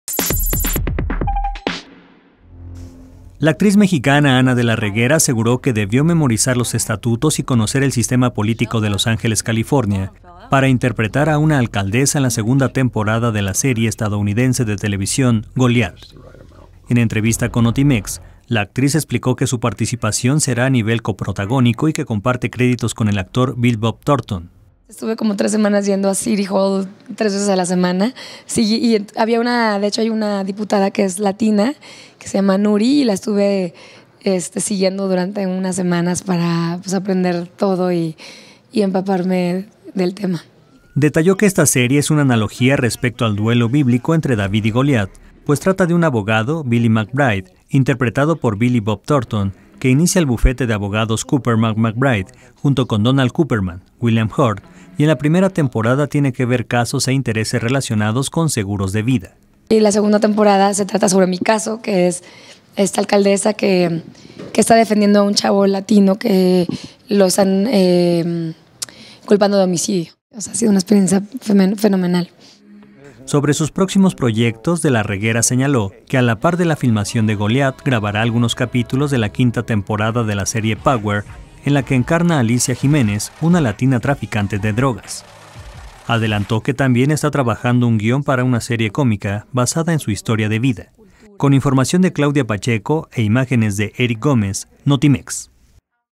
Ana de la Reguera interpretará alcaldesa latina en serie en EU
La actriz señaló que previo a la filmación se preparó estudiando los estatutos y el sistema político de Los Ángeles, California